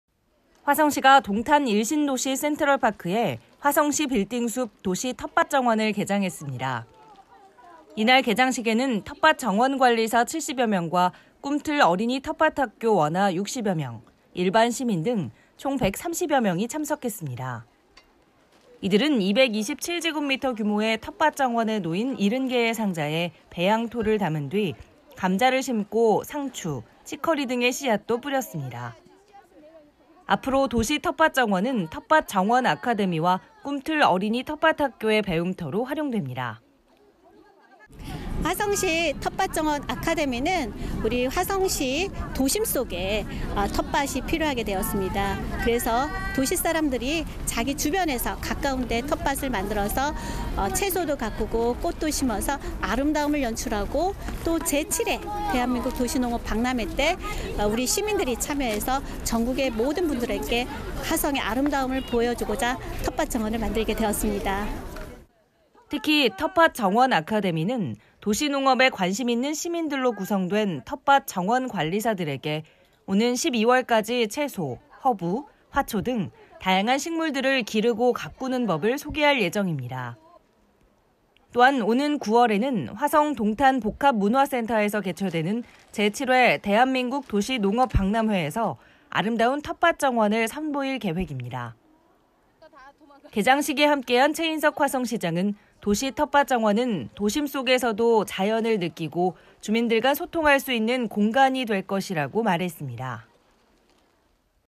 화성시 동탄 도시 텃밭정원 개장
화성시가 동탄 1신도시 센트럴파크에 '화성시 빌딩숲 도시텃밭정원'을 개장했습니다. 텃밭정원관리사 70여명과 꿈틀 어린이 텃밭학교 원아 60여명, 일반시민 등 총 130여명이 참석했습니다.